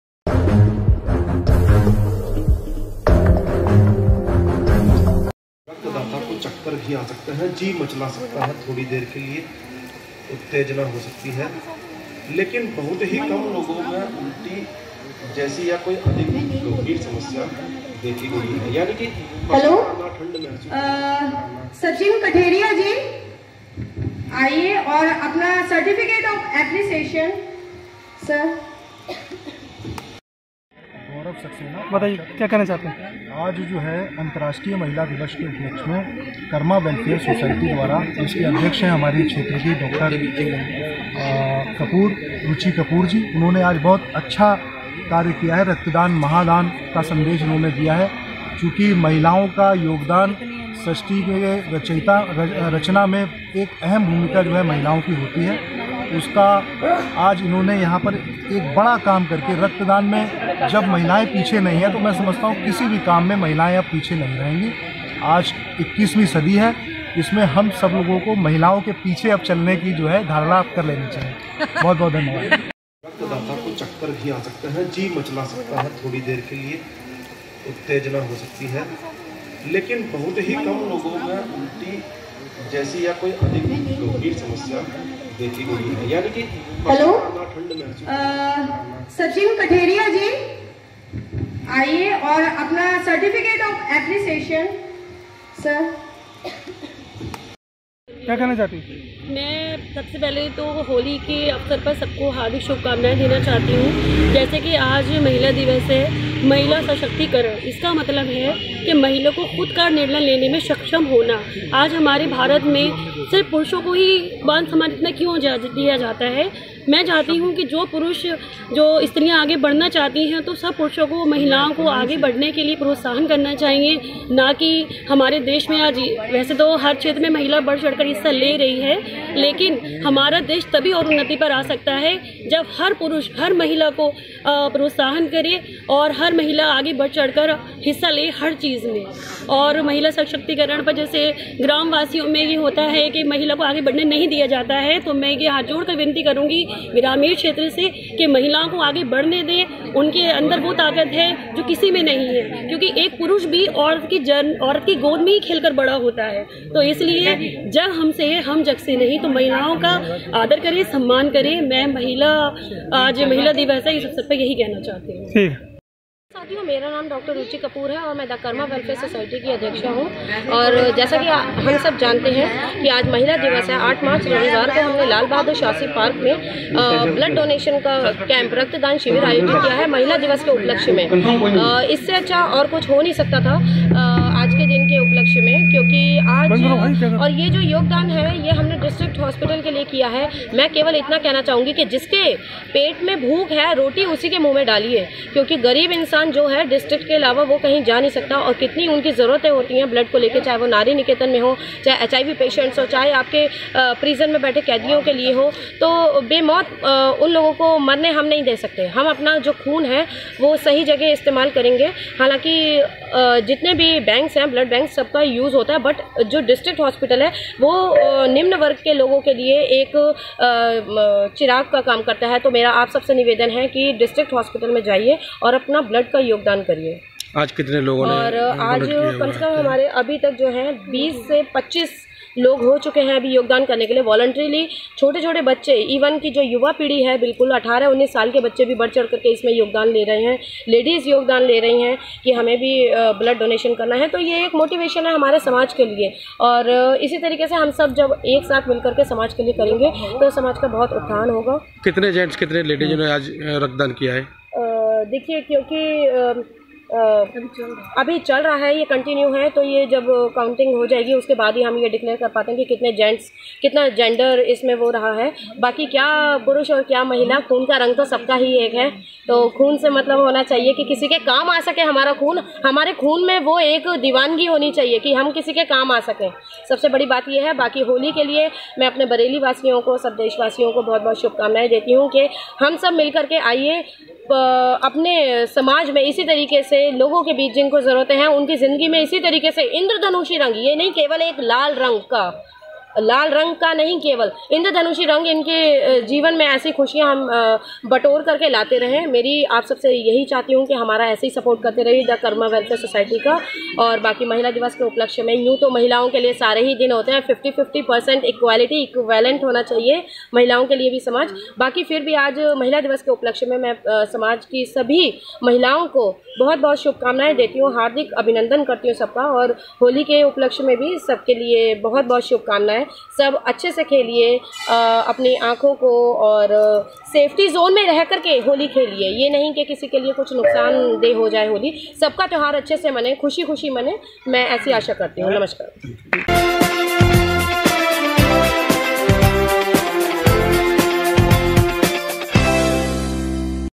रुचि कपूर कपूर के नेतृत्व में महिला दिवस के मौके पर शास्त्री नगर में रक्तदान किया गया
दी कर्मा वेलफेयर सोसाइटी के अध्यक्ष डॉ रुचि कपूर  कपूर के नेतृत्व में महिला दिवस के मौके पर शास्त्री नगर में रक्तदान किया गया जिसमें सोसाइटी के सभी सदस्यों के साथ 17 और लोगो ने रक्तदान किया इस मौके पर  अंजना सक्सेना ,इमालदा परवीन,  सुभि, विवेककपुर,अयूब सक्सेना  सकुन्तला जी अंशुल खन्ना , सभसाद गौरव सक्सेना अदि लोग मौजूद रहे